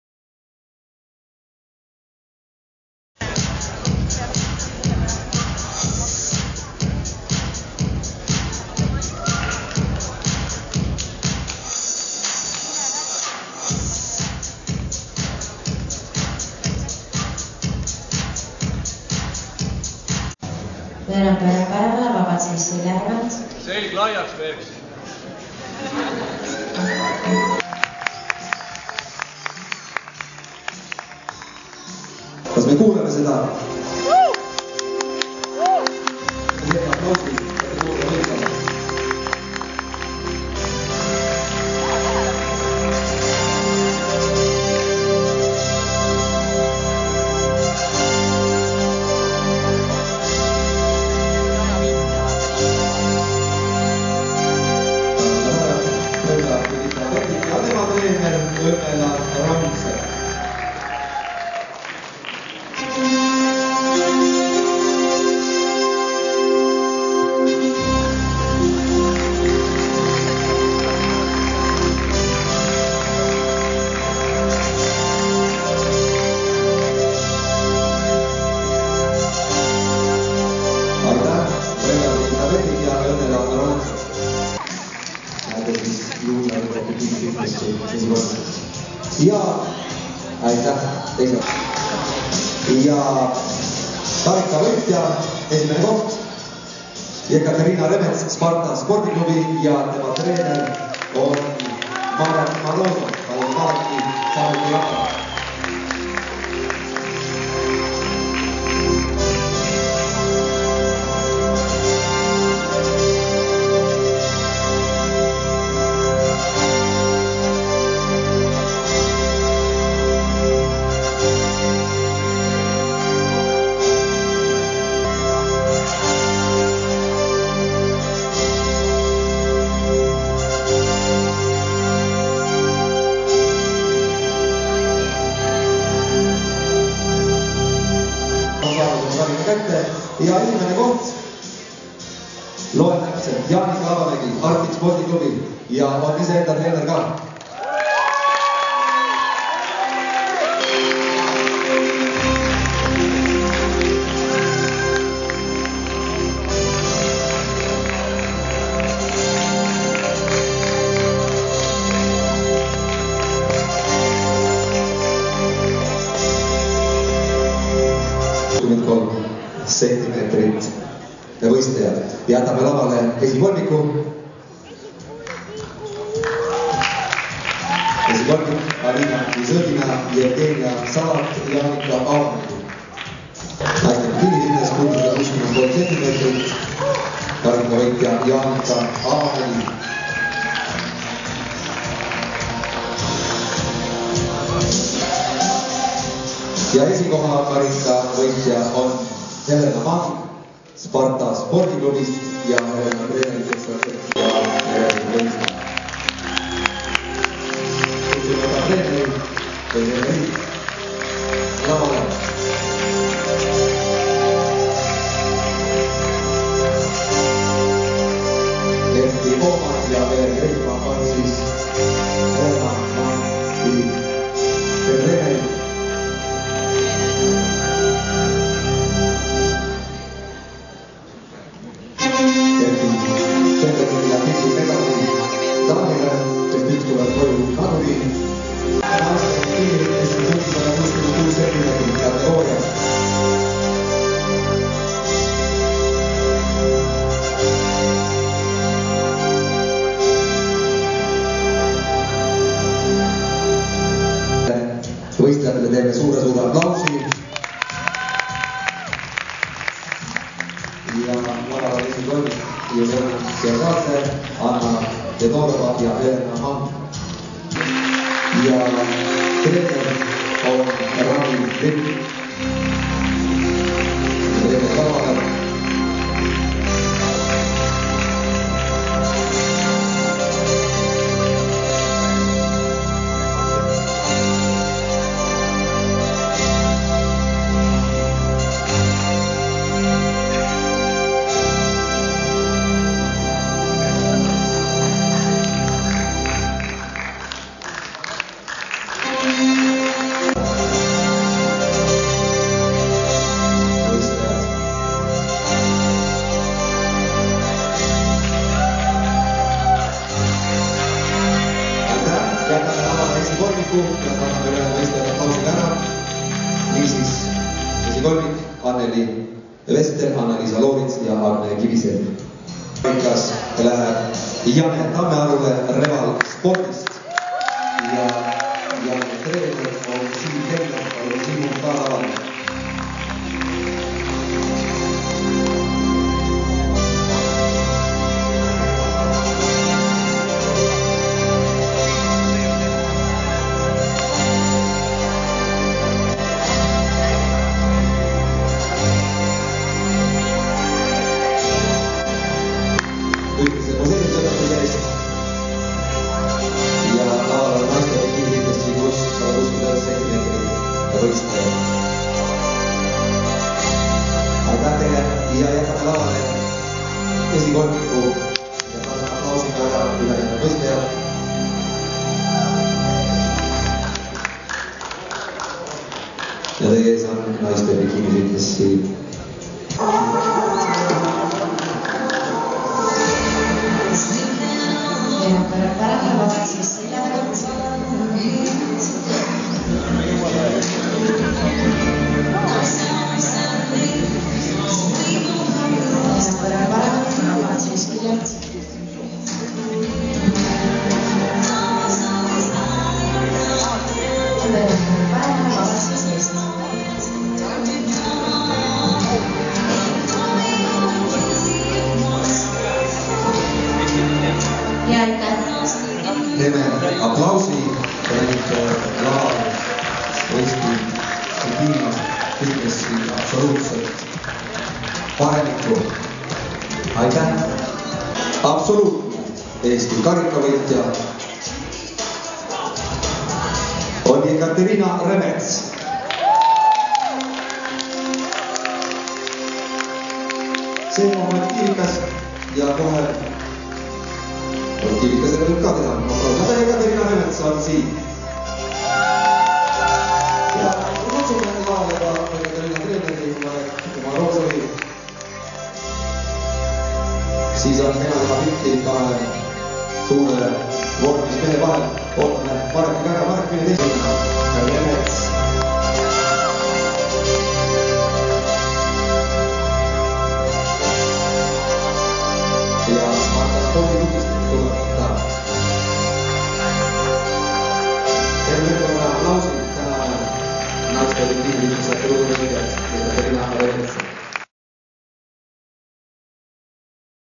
Best bikini fitness 2017 Eesti karikavõistlused. fotoViktorBurkivski
video  Best bikini fitness 2017 Eesti karikavõistlused. Tallinn . 29. aprillil toimusid Salme Kultuurikeskuses Eesti karikavõistlused kulturismis ja fitnessis.
Enim võistlejaid oli bikiinifitnessi erinevates kategooriates:
Noorte bikiinfitnessi karikavõitja tiitli võttis Brenda Brigitta Vettik (HC Gym)
Juunioride kategooria võitjaks tuli Arida Muru (Arctic Sport Club), 2. koht Julia Tšižova (HC Gym), 3. koht Liis Marian Toomse (Arctic Sport Club).
Mastersite arvestuses oli parim Mari Pukk (Tallink Fitness), 2. koht Julija Želobetski (MyFitness), 3. koht Julia Smoli (Sparta Spordiklubi)
Kõige lühema bikiinifitnessi kategooria, -160cm võidu võttis Jekaterina Remets (Sparta Spordiklubi), 2. koht Katarina Kallikorm (HC Gym), 3. koht Merili Jesaar (Sparta Spordiklubi).
-163cm võitjaks tuli Janika Aavamägi (Arctic Sport Club), 2. koht Jevgenia Salak (HC Gym) ning 3. koht Alina Buzõgina (HC Gym)
-166cm võit läks Helena Mangile (Sparta Spordiklubi) ning pikkuskategooria, 2. koht Anna Fedorova (MyFitness), 3. koht Julianna Giorgadze (Arigato Spordiklubi)
-169cm esimese koha võttis Agne Kiviselg (Spa&Sport), 2. koht Anna-Liisa Loorits (Sparta Spordiklubi), 3. koht Anneli Vestel (MyFitness).
Enim võistlejaid oli bikiinifitnessi pikkuskategoorias +169cm, kus tuli võitjaks Jane Tammearu (Reval-Sport), 2. koht Nele Narjanen (Arigato Spordiklubi), 3. koht Betti Nigul (Tallink Fitness).
Absoluutselt parimaks bikiinifitnessis kuulutati Jekaterina Remets.